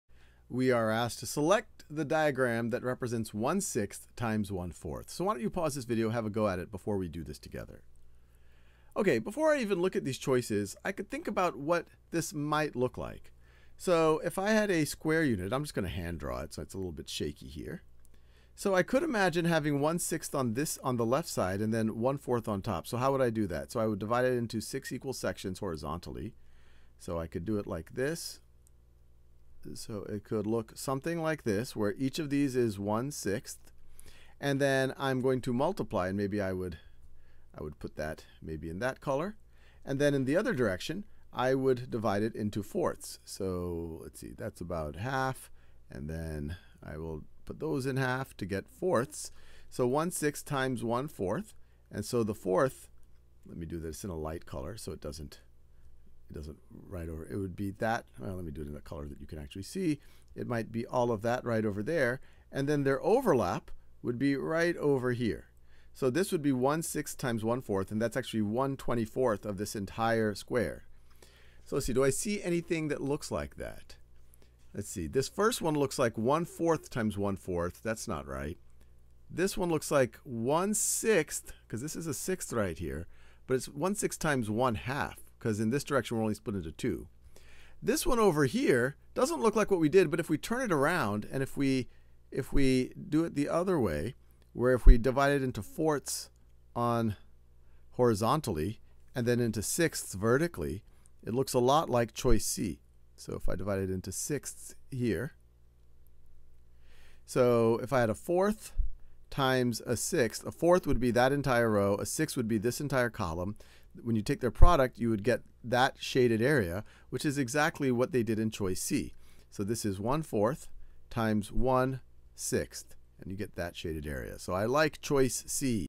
Multiplying unit fractions: expression to area model | Khan Academy
Worked example on how to represent a multiplication expression involving a unit fraction and a unit fraction with an area model.